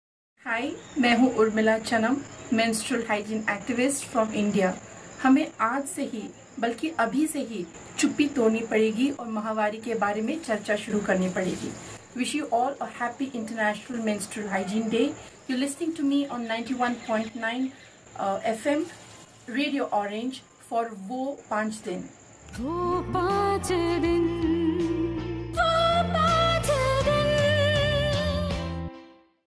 WO 5 DIN | MENSTRUATION HYGIENE MANAGEMENT | URMILA CHANAM | MENSTRUAL HEALTH ACTIVIST
Our Sincere Thanks to Menstrual Health Activist & Founder 'Breaking the Silence' Campaign from Banglore Urmila Chanam for her kind support in Wo5Din Activity...
Water Supply & Sanitation Collaborative Council (WSSCC)